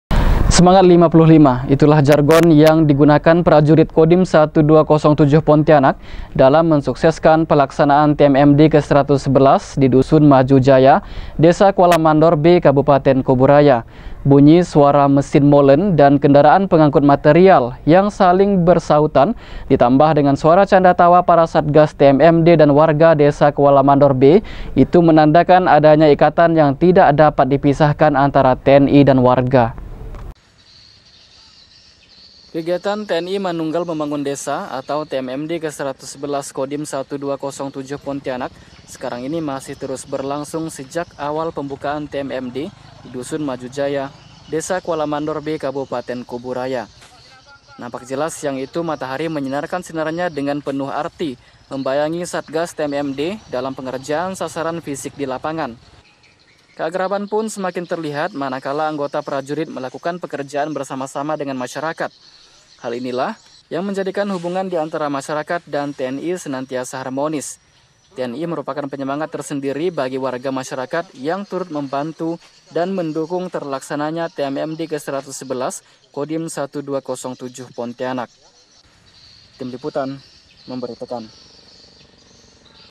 SEMANGAT 55 PRAJURIT DALAM PELAKSANAAN TMMD
Semangat 55, itulah jargon yang digunakan Prajurit Kodim 1207 Pontianak dalam mensukseskan pelaksanaan TMMD ke-111 di Dusun Maju Jaya, Desa Kuala Mandor B, Kabupaten Kubu Raya. Bunyi suara mesin molen dan kendaraan pengangkut material yang saling bersautan ditambah dengan suara canda tawa para satgas TMMD dan Warga Desa Kuala Mandor B, itu menandakan adanya ikatan yang tidak dapat dipisahkan antara TNI dan warga